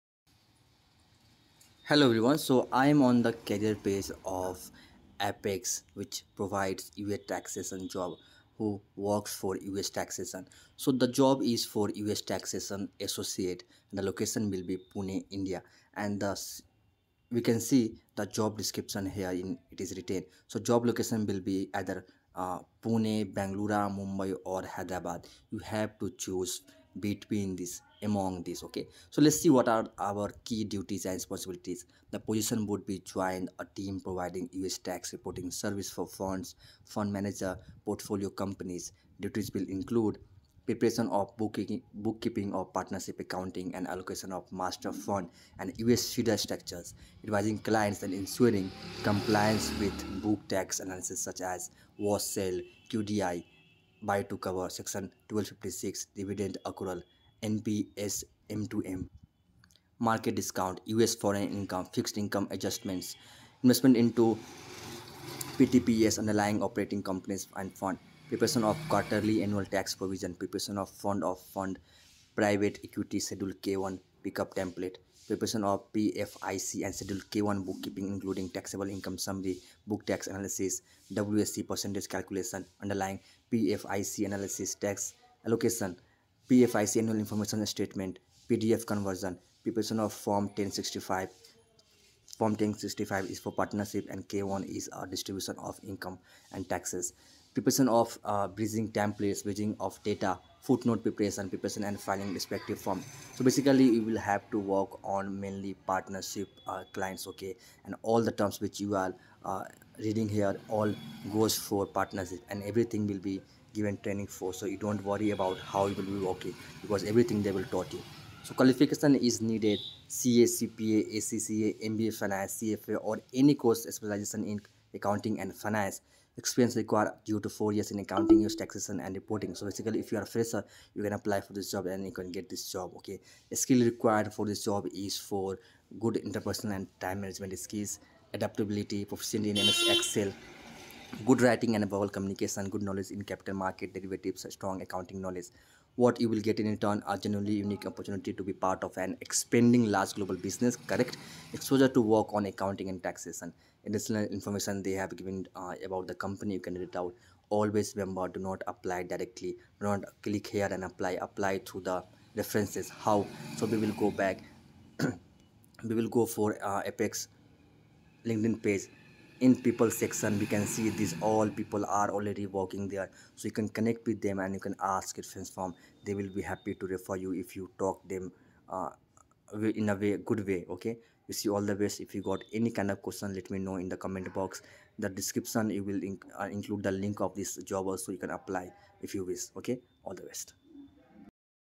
US Tax Associate for freshers| Best job for finance freshers
Key duties and responsibilities

The position would be to join a team providing US Tax Reporting Services for Funds, Fund Managers and Portfolio Companies.  Duties would include:

Preparation of Book-Keeping of Partnership accounting and allocations for Master funds and US Feeder structures
Advising clients and ensuring compliance with Book-Tax Analyses such as:
- Wash Sales
- QDI/DRD
- Buy to Cover
- Section 1256
- Dividend accruals
- NPC MTM
- Market discount
- US/Foreign income
- Fixed Income adjustments (amortization/accretion)
- Straddles
- Investment into PFICs, PTPs, Underlying Operating Companies and Funds
Preparation of Quarterly/ Annual Tax Provision.
Preparation of Fund of Fund/Private Equity Schedule K-1 Pickup template
Preparation of PFIC and Schedule K-1 workbook including:
Taxable Income Summary
Book/tax analyses
WAC % calculation
Underlying PFIC analyses
Tax Allocations (aggregate, FoF, Private Equity)
PFIC Annual Information Statement and cover letter preparation
PDF conversion
Preparation of Form 1065/Schedule K-1 Preparation via GoRS
Preparation of bridging templates
Bridging of data
Footnote preparation
Preparation and filing of respective forms
Qualification and Experience

Qualification- CA, CPA, ACCA, MBA (Finance), CFA, or any course specialized in Accounting/Finance.
Experience of 0-4 years in Accounting, US Taxation and Reporting
Skills Required

Good interpersonal and time management skills
Adaptability
Proficient in MS Excel and MS Word
Good written and verbal communication skills
Good knowledge in capital market and derivatives
Strong accounting knowledge

Want to connect for one to one call on telegram for free? Join 👇

Want to connect professionally for free? Join